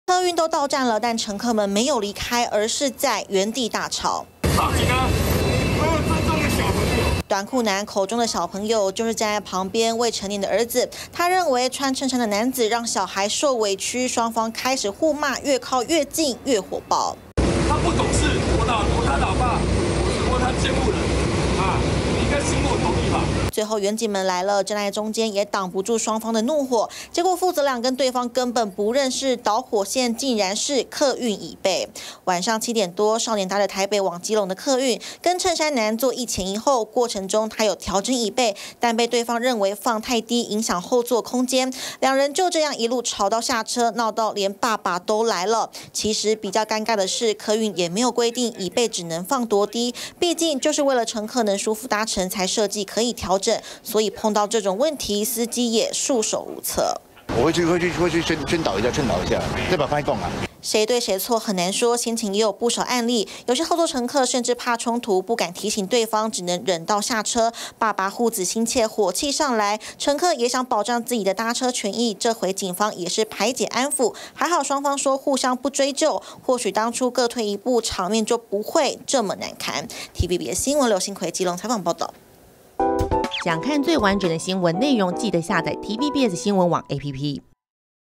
「你椅背放太低」 父為護兒槓乘客 怒咆哮｜TVBS新聞
一名少年，從台北搭客運要基隆家時，因為調整椅背，被後座乘客認為放太低，影響到空間，兩人爆發口角，一路爭執到站，最後連少年爸爸都來了，雙方就在路邊大吵！